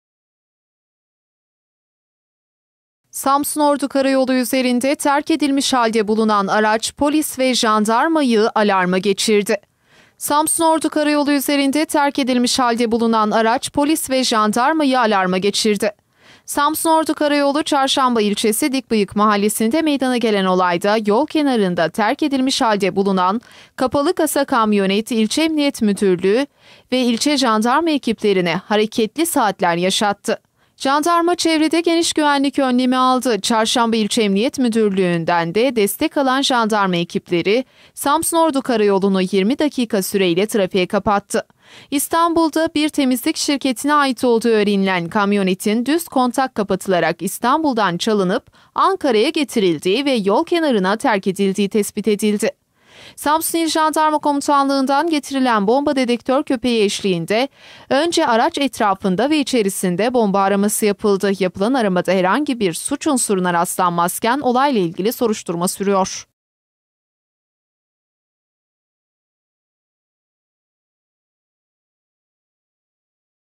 KARAYOLUNDA BOMBA PANİĞİ
Samsun-Ordu karayolu üzerine terk edilen 34 plakalı kapalı kasa kamyonet 'bomba paniği' yaşattı. Polis ve jandarma ekipleri yolu trafiğe kapatarak aracı incelemeye aldı.